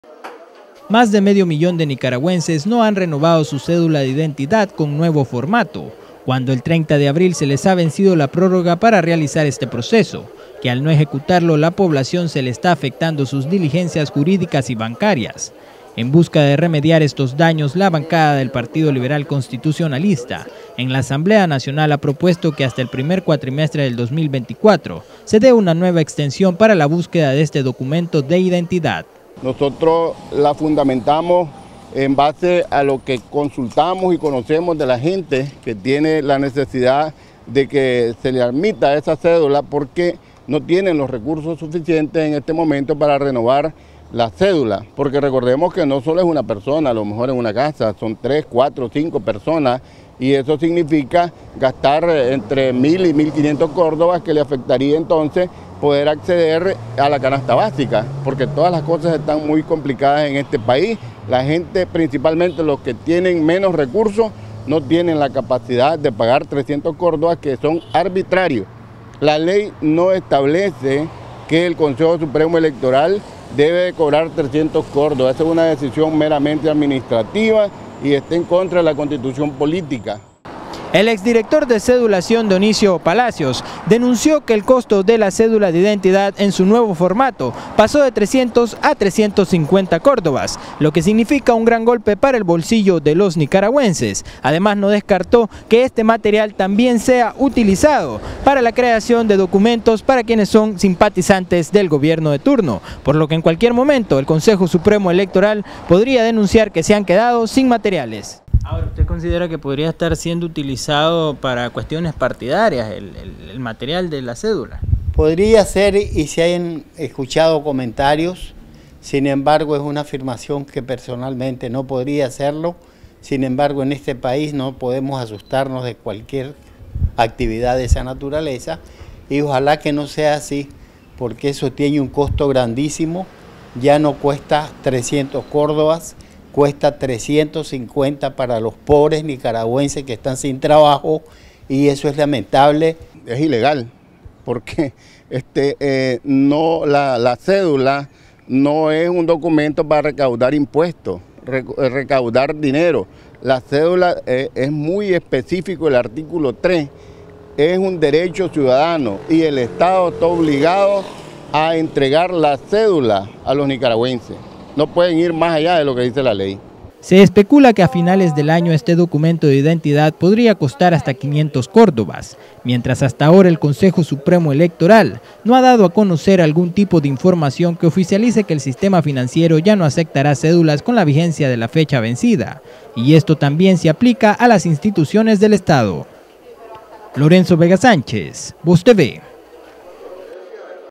CSE podría quedarse sin material para cédula de identidad por esta razón
Más de medio millón de nicaragüenses no han renovado su cédula de identidad con nuevo formato. El 30 de abril se venció la prórroga para realizar este proceso, al no ejecutarlo a la población se le está haciendo difícil hacer sus diligencias jurídicas y bancarias.